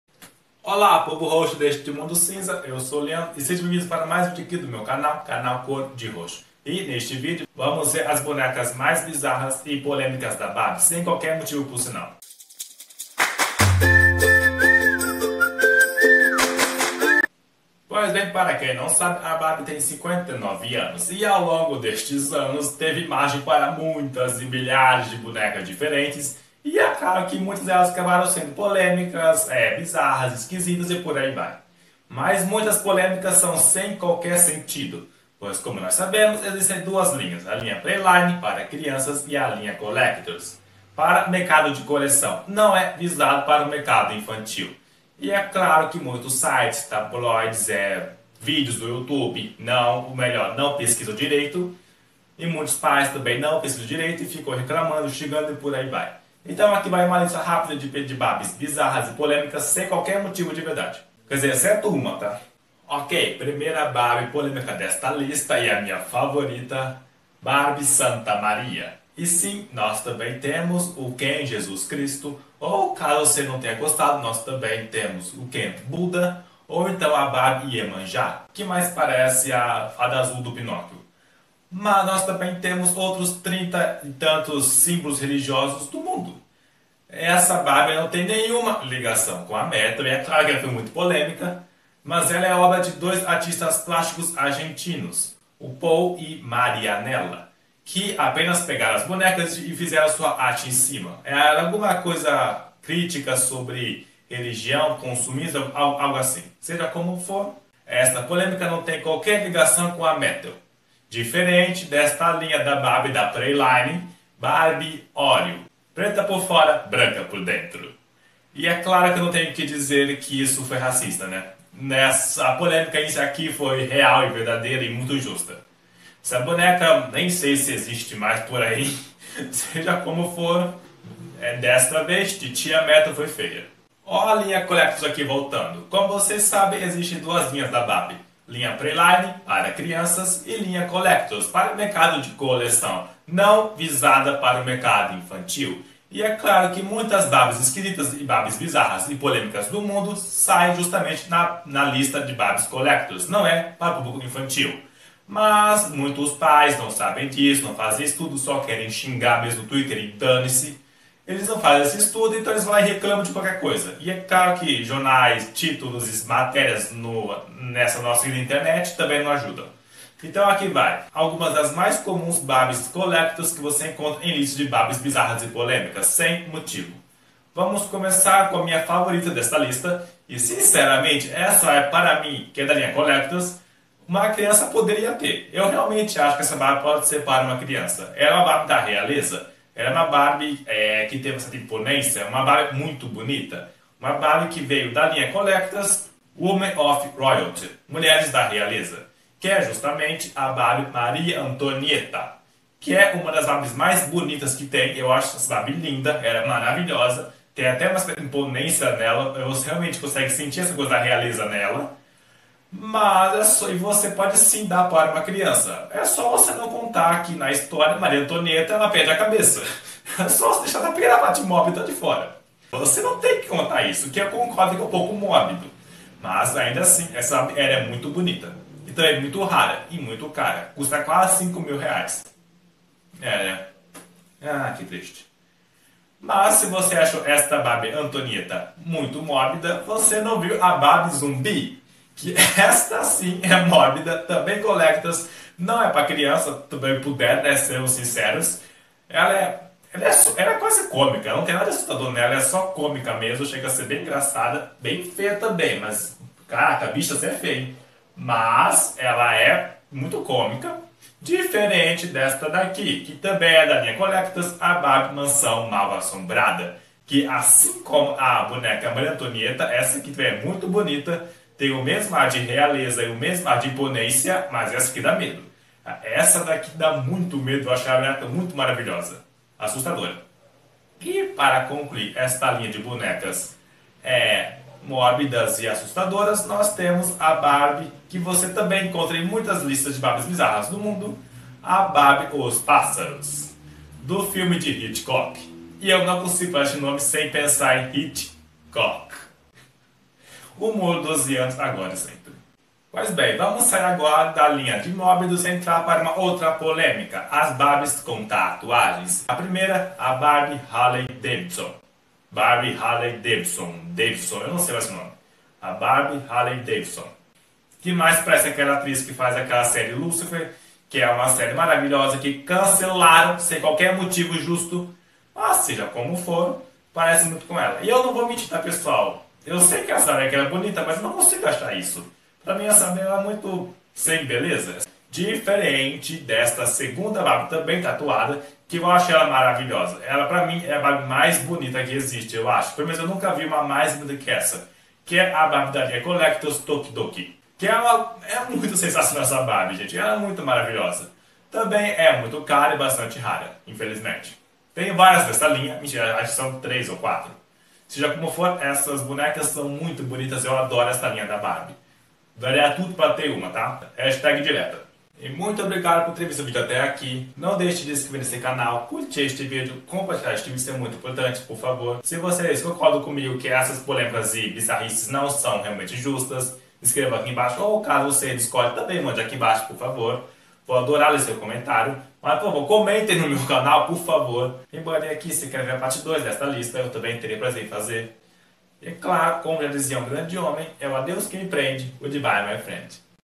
10 BARBIES (NEM TÃO) MAIS BIZARRAS E POLÊMICAS
10 Barbies bizarras, mas nem tanto, e polêmicas, e também, sem motivos. As criticas mais sem sentido, provas que ninguém pesquisa de verdade antes de reclamar. Exceto um caso aqui.